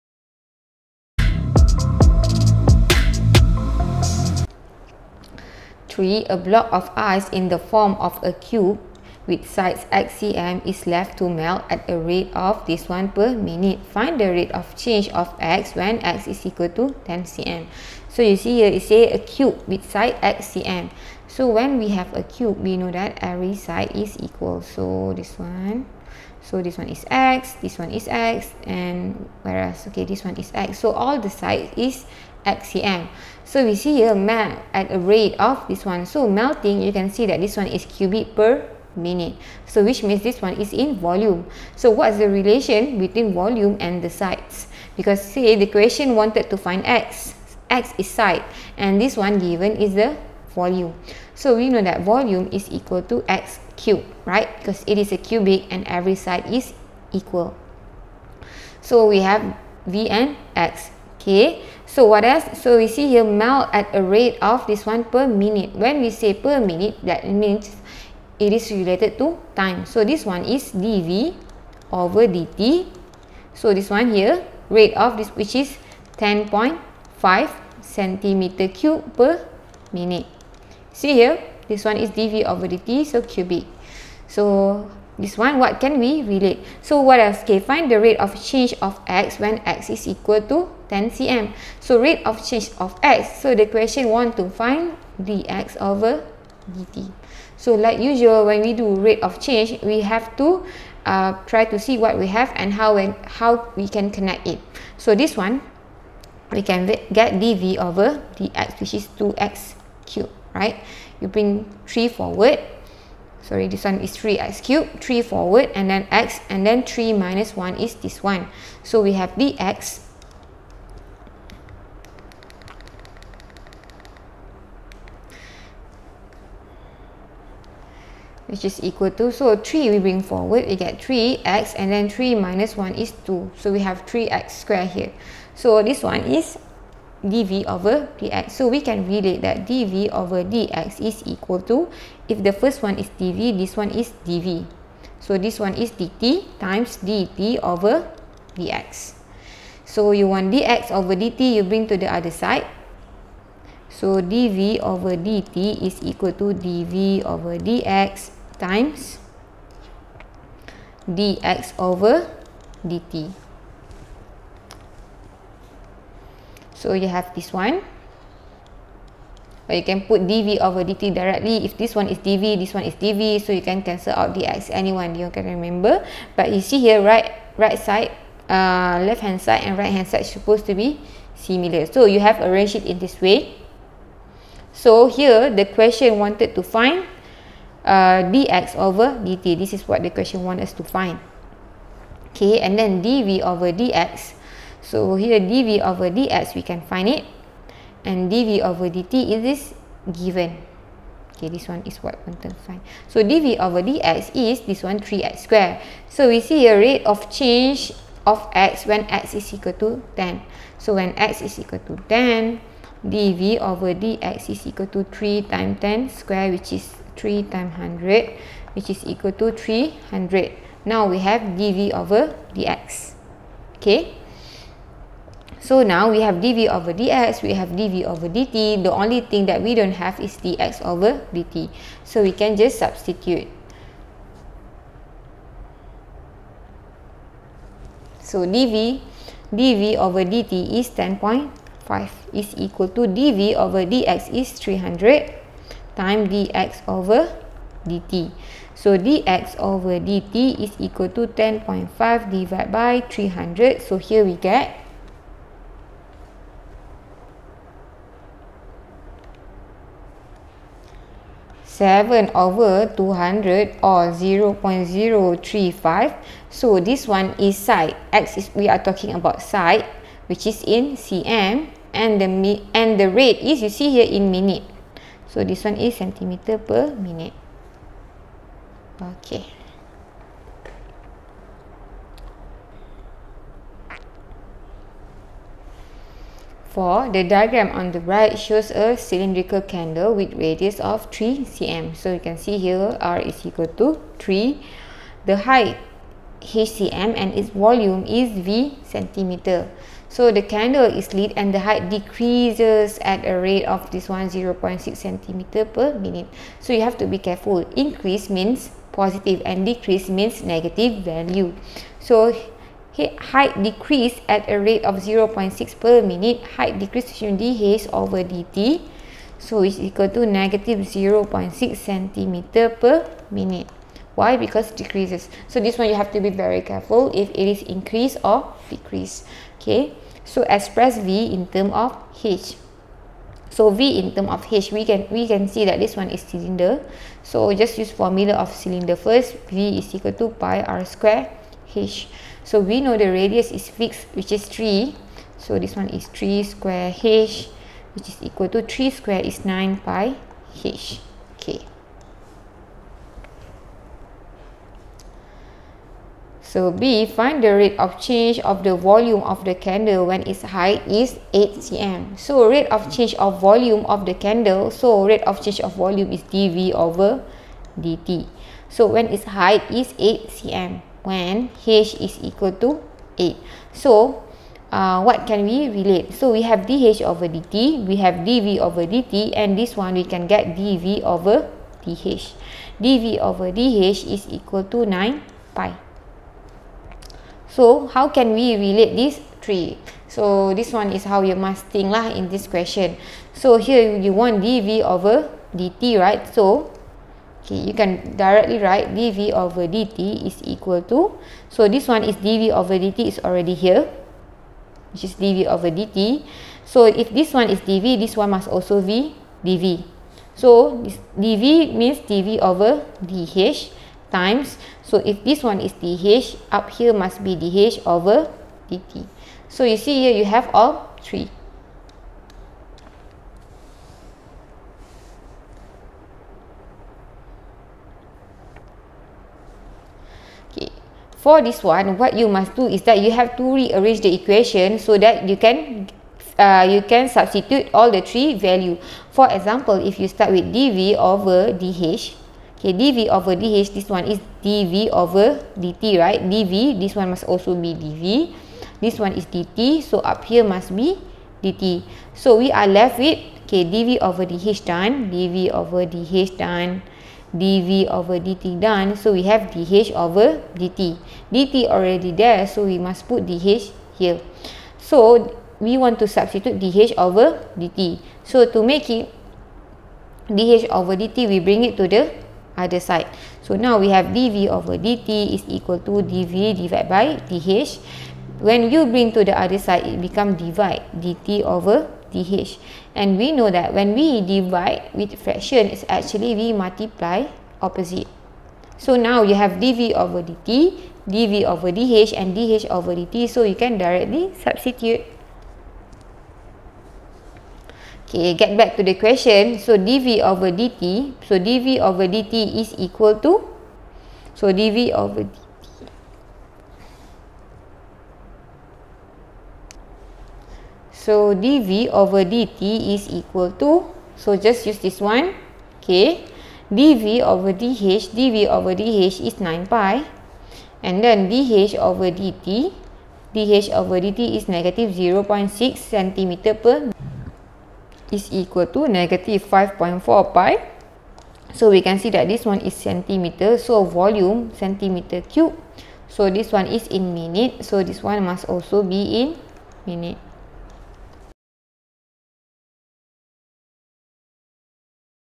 Differentiation self-exercise 2.13 Q3 Q4 pembezaan latihan kendiri 2.13 form 5 add maths kssm spm
Differentiation self-exercise 2.13 Q3 Q4 pembezaan latihan kendiri 2.13 form 5 add maths kssm spm form 5 textbook  add maths buku teks matematik tambahan

3. A block of ice in the form of a cube with sides x cm is left to melt at a rate of
 10.5 cm3 per minute. Find the rate of change of x when x = 10 cm.

4. The diagram on the right shows a cylindrical candle
 with radius 3 cm. The height is h cm and its volume is
V cm3. The candle is lit and the height decreases at a
rate of 0.6 cm per minute.
(a) Express V in terms of h.
(b) Find the rate of change of the volume of the candle
 when its height is 8 cm.